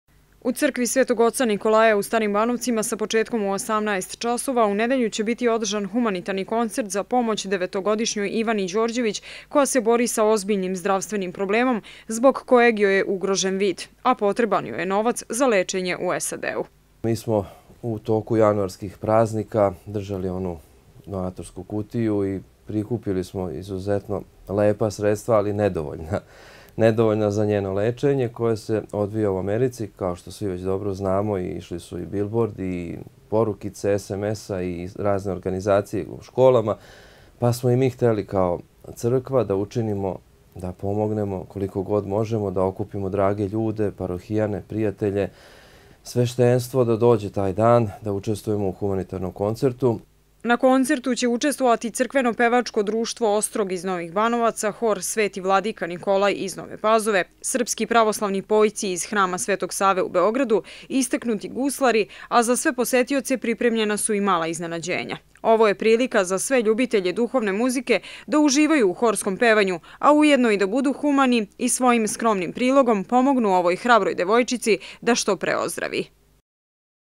Najava humanitarnog koncerta za Icu
U nedelju 28. februara u starobanovačkoj crkvi biće održan humanitarni koncert za pomoć devetogodišnjoj Ivani Đorđević iz ovog mesta.